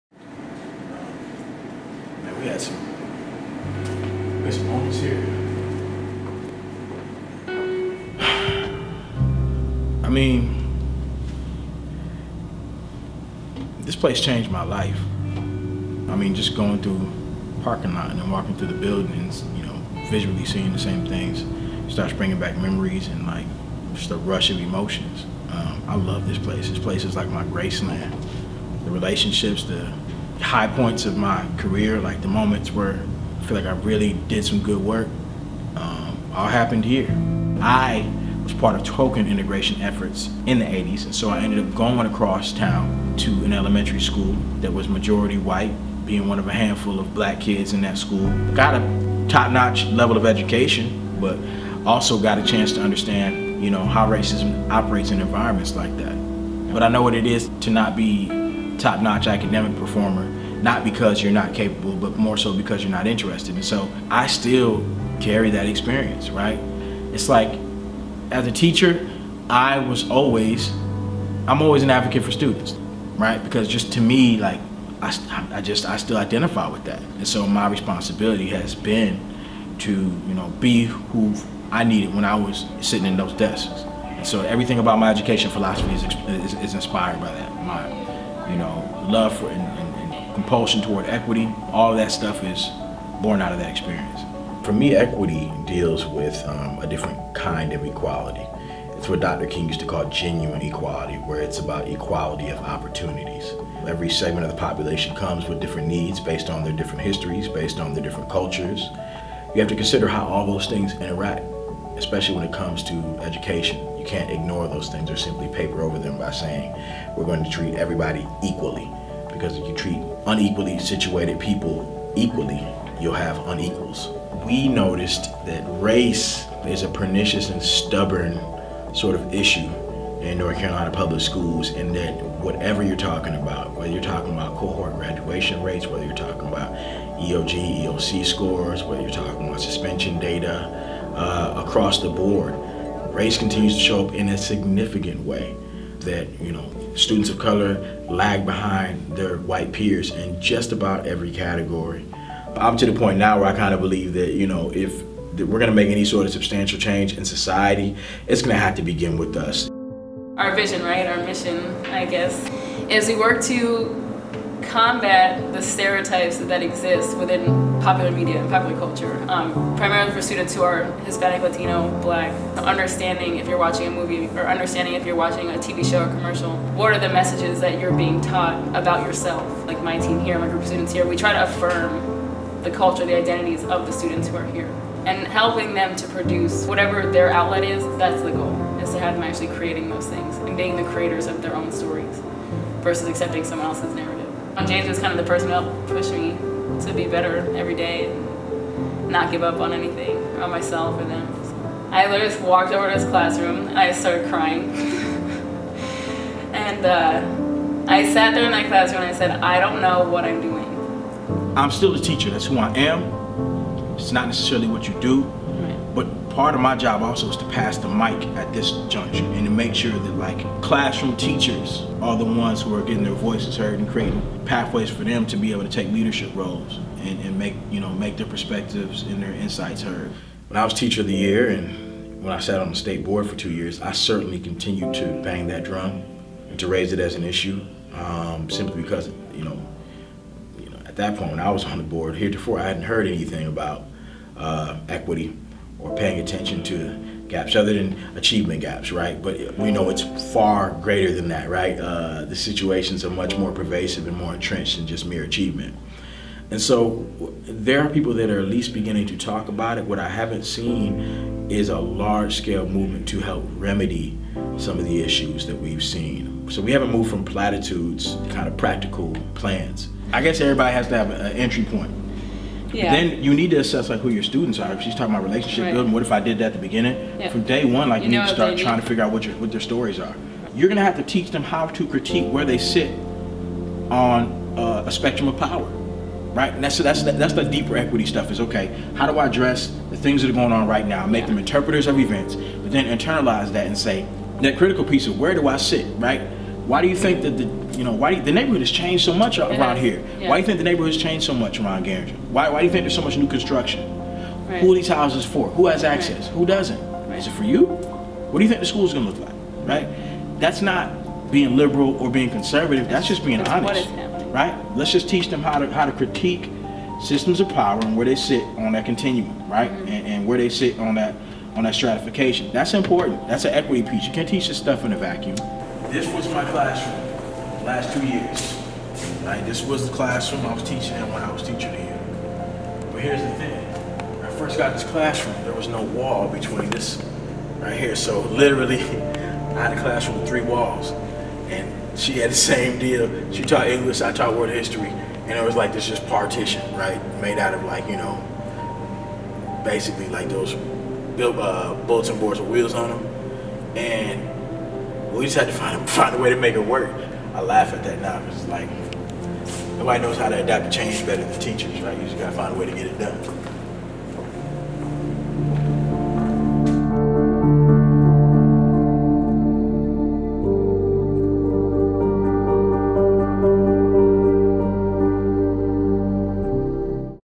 Equity Meets Education: James Ford
This film is the last in a four-part series exploring racial equity in North Carolina schools through the lives and perspectives of some of the state’s black male leaders. James Ford, 2014 North Carolina Teacher of the Year and principal at Filling the Gap Educational Consultants, tells us how his childhood and teaching experience inform his role in fighting for educational equity.